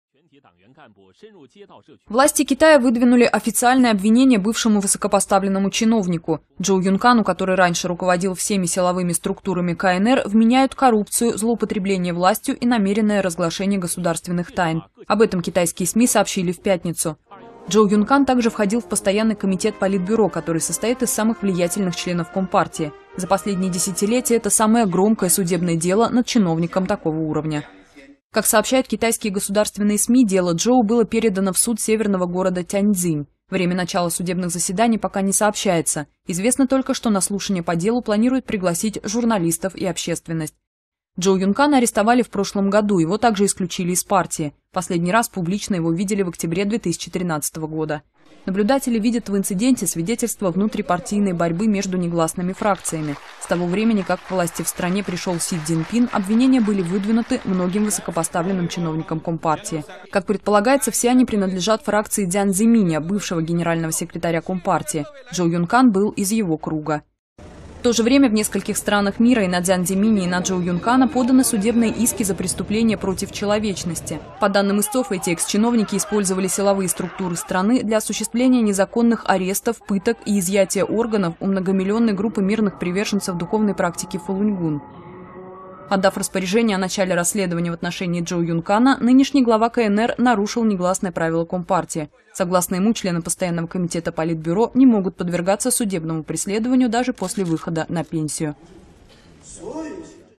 Против экс главы силовых структур КНР выдвинули обвинения
Власти Китая выдвинули официальные обвинения бывшему высокопоставленному чиновнику. Чжоу Юнкану, который раньше руководил всеми силовыми структурами КНР, вменяют коррупцию, злоупотребление властью и намеренное разглашение государственных тайн.